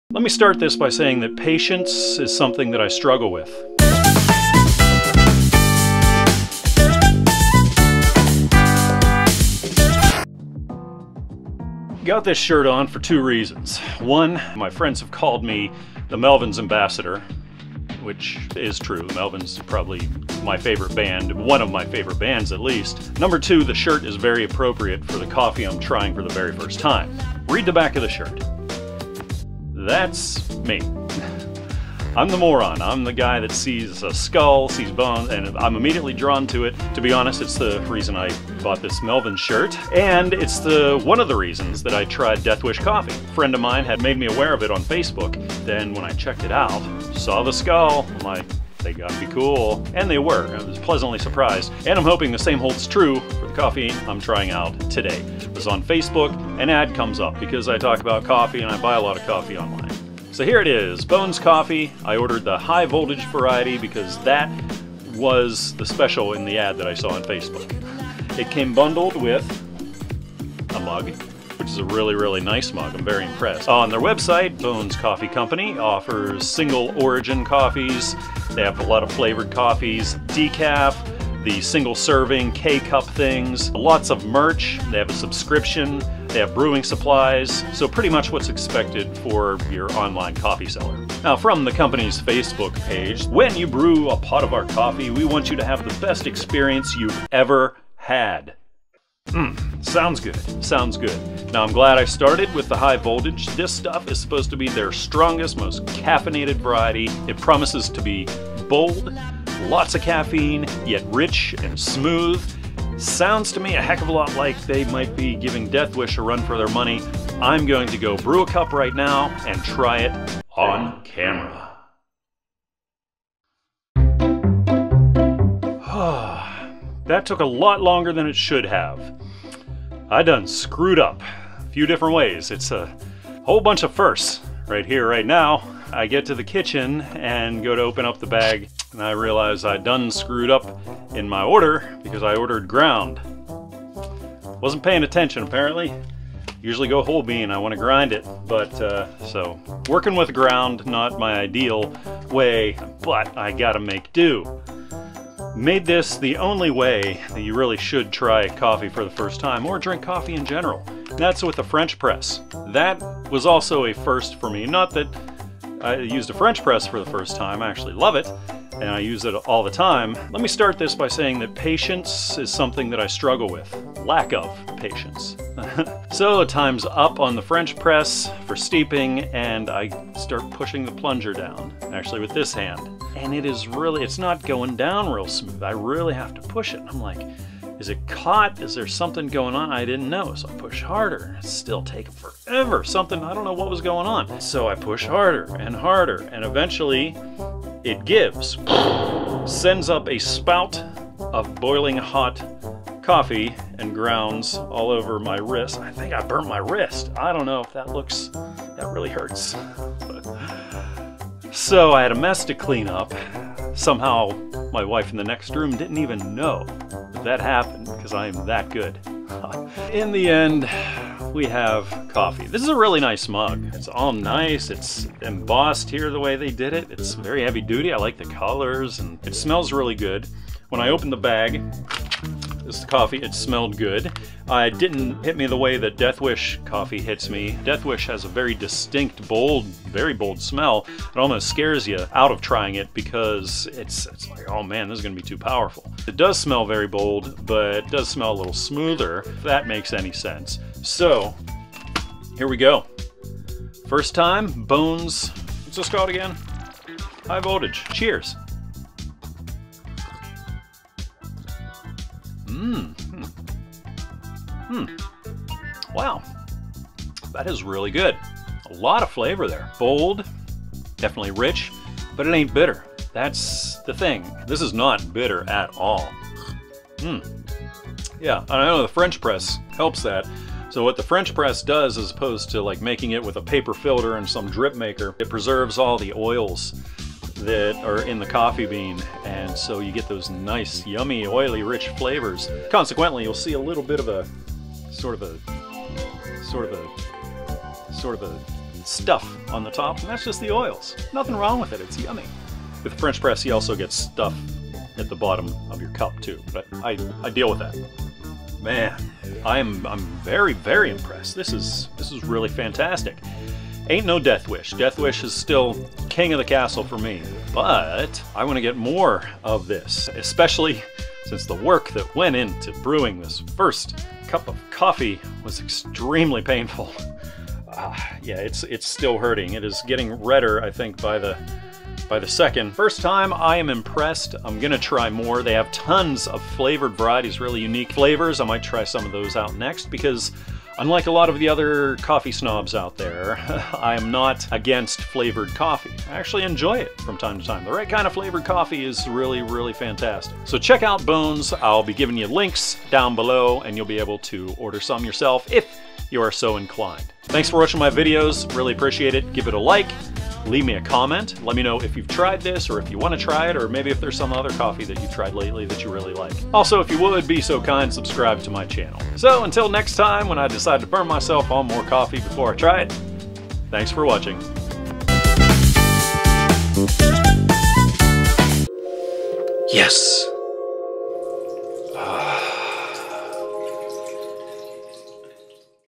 Bones Coffee | INJURIES SUSTAINED
The day I decide to try Bones High Voltage Coffee, my french press attacks me.